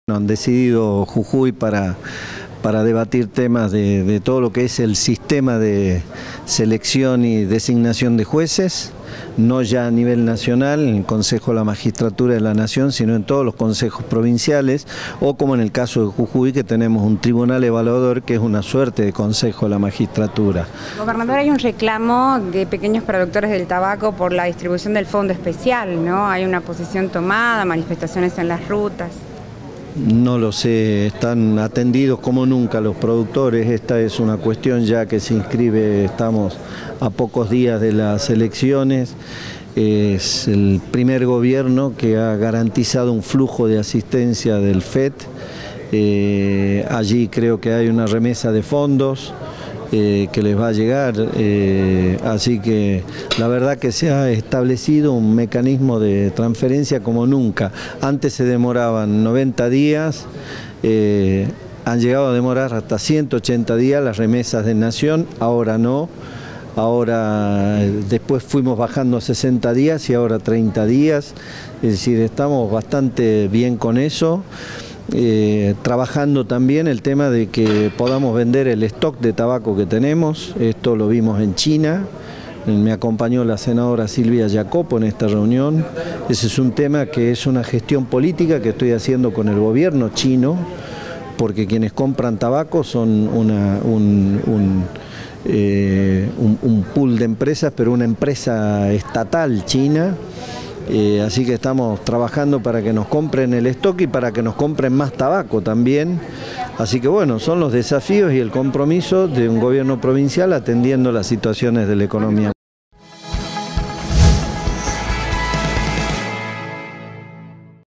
Respaldo al sector tabacalero de Jujuy
El gobernador participó del acto de inicio de la nueva campaña de acopio en la Cooperativa de Tabacaleros de Jujuy.